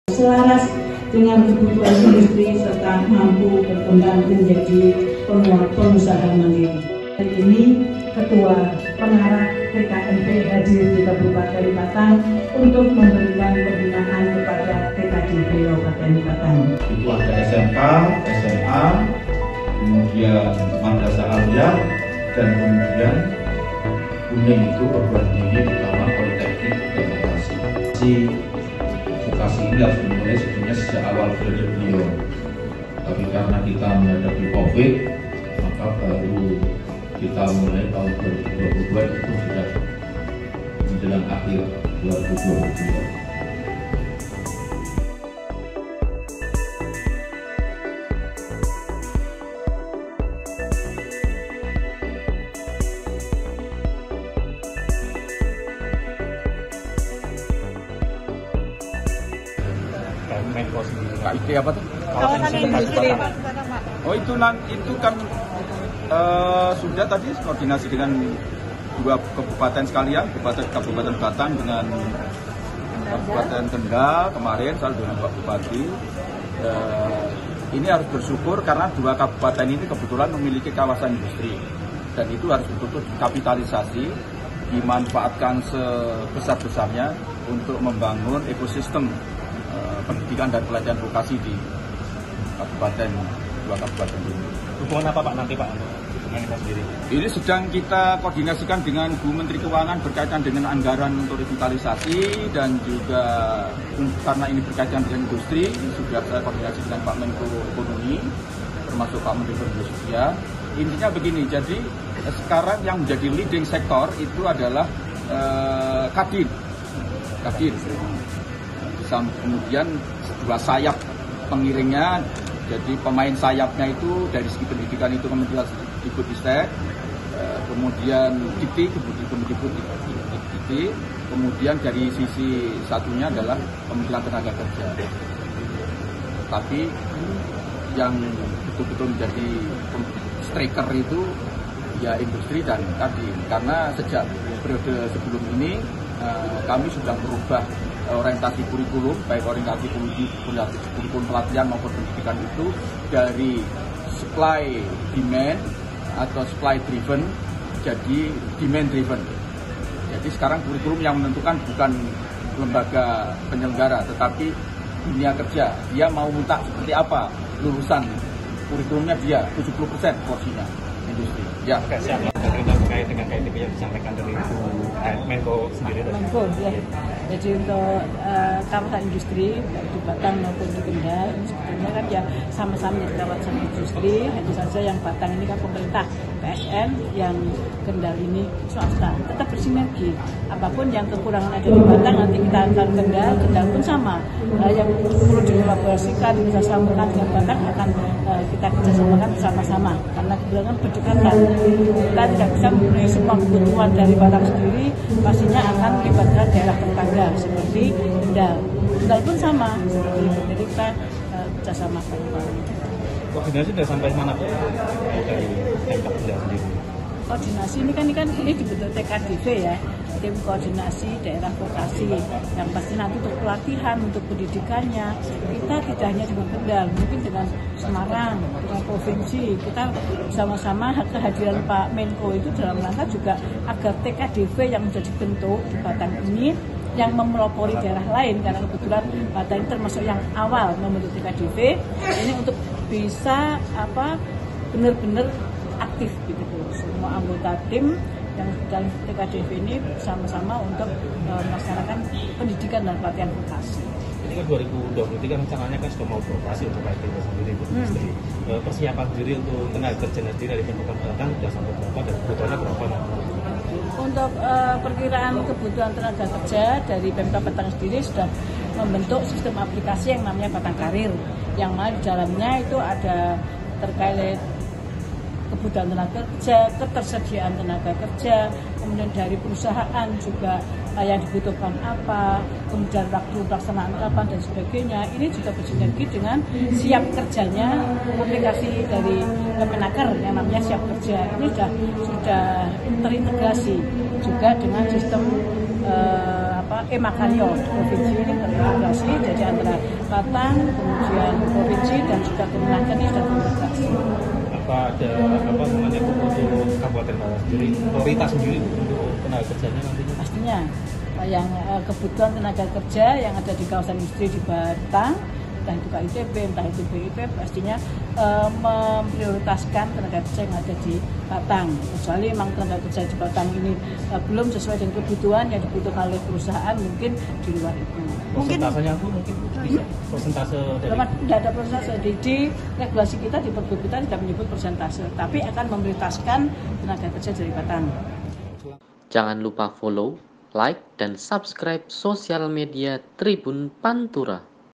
Muhadjir Effendy Bentuk TKDV di Batang
Selengkapnya Menteri Koordinator Bidang Pembangunan Manusia dan Kebudayaan (PMK), Muhadjir Effendy membentuk Tim Koordinasi Daerah Revitalisasi Pendidikan dan Pelatihan Vokasi (TKDV) di Aula Kantor Bupati Senin (13/3/2023). Hal itu mengacu pada Peraturan Presiden (Perpres) Nomor 68 Tahun 2022 tentang Revitalisasi Pendidikan Vokasi.